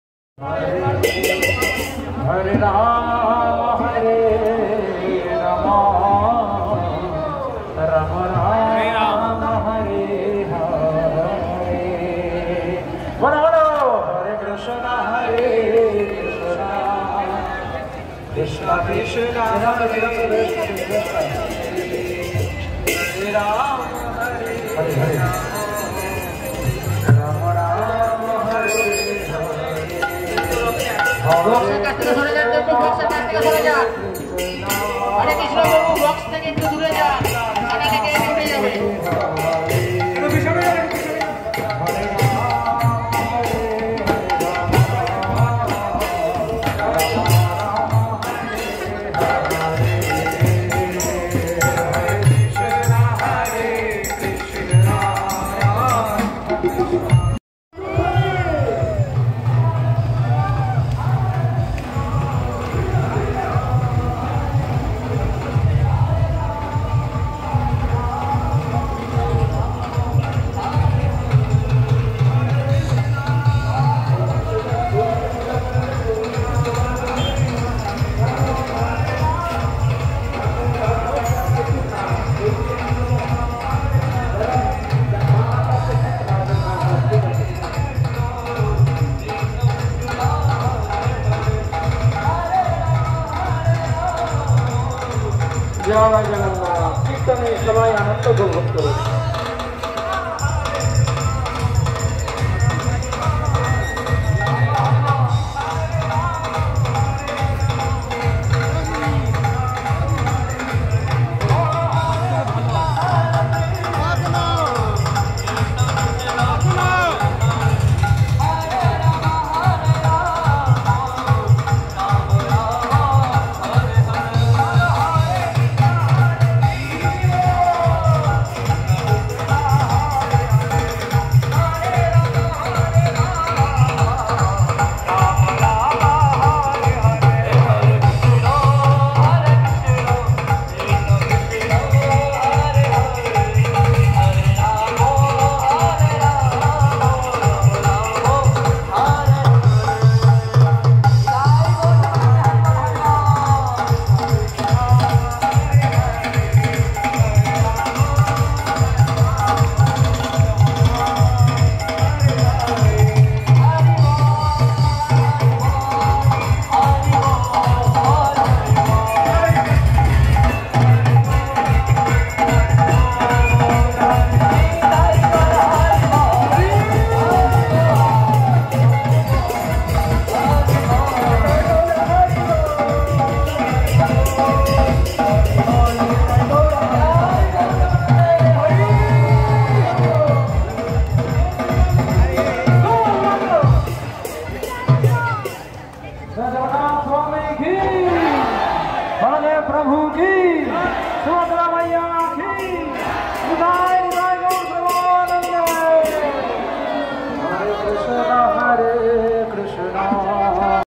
Kolkata Iskcon Rathayatra Ultorath at Esplanade yesterday. Enjoy.
Edited by VideoGuru কলকাতায় উলটোরথের যাত্রা ইস্কন মন্দিরের। ধর্মতলা চত্তর থেকে ভক্তেরা দেশি ও বিদেশিনীরা চলেছেন কৃষ্ণ নাম করতে করতে।